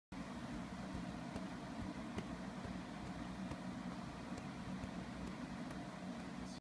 Dog Wags Tail on other Dog's Head
This dog tries so hard to have a peaceful nap that he doesn't let anything, even his sibling dog wagging his tail right on his head, stop him from his relaxation.